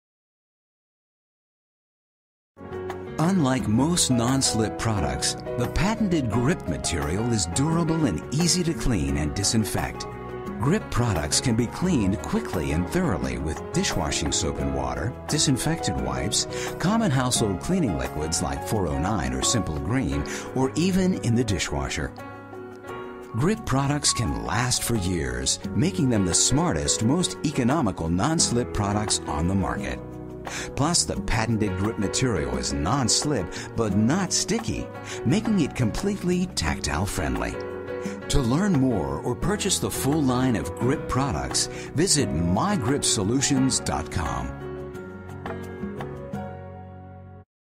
GRIP Solutions Care & Cleaning
Unlike most non-slip products, the patented GRIP™ material is durable and easy to clean and disinfect. GRIP™ products can be cleaned quickly and thoroughly with dishwashing soap and water, disinfectant wipes, common household cleaning liquids, or even in the dishwasher! GRIP™ products can last for years, making them the smartest, most economical non-slip products on the market.